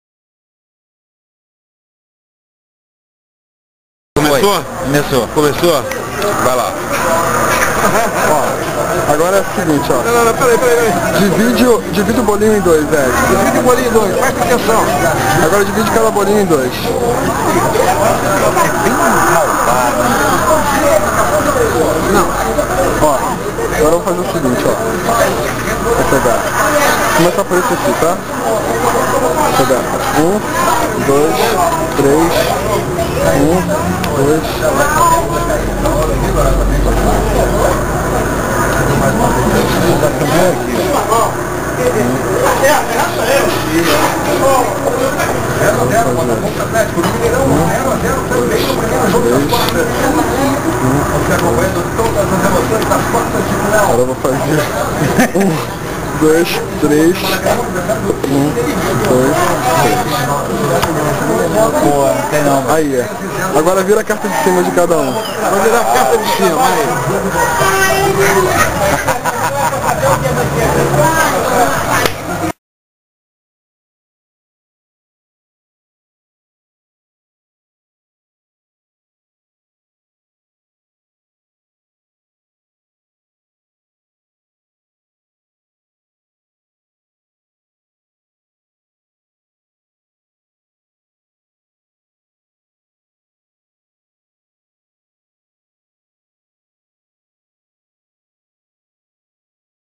fragmentos de bar - o mágico
o mágico Aipim apresenta um de seus famosos truques com baralho. No bar do italiano em botafogo